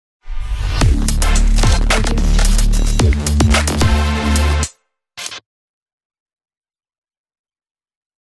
Glitch Typo Logo After Effects Templates
Description
Meet our new, incredibly energetic typo logo opener from Proskurovskiy team. Inspired by the aesthetics of the early 2000s, we created this glitch opener for those who are not afraid of bold solutions and want to leave a trace in the memory of everyone who sees your showreel, promo or YouTube channel for a long time. Place your logo, change texts and choose colors very easy — thats all you need to get your unique intro in a minutes!
Basic Features:
1 Logo Placeholder
6 Textholders
Works without any plugins
Full HD
Fast Render
Duration 0:08
Easy to customize
Font – OpenSans & VCR OSD